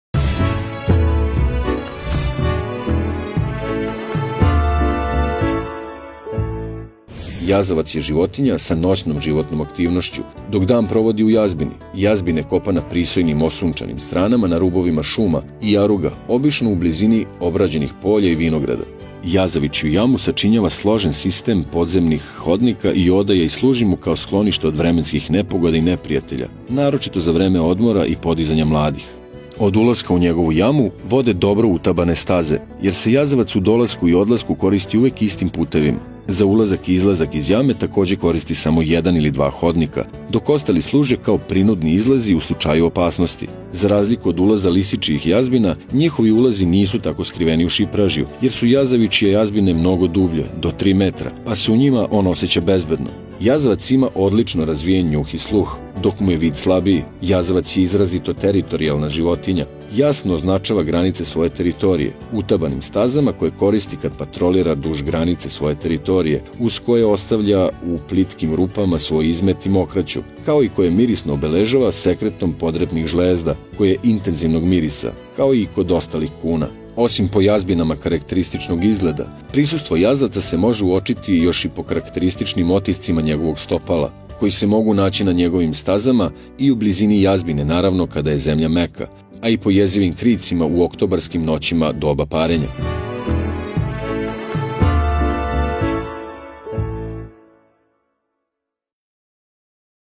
Lov na jazavca - Lovački saveti
Za odstrel jazavca se koristi puška sačmarica standardnih kalibara 12, 16 i 20 i širokih čokova jer je jazavac spora životinja na koju se puca na malim rastojanjima.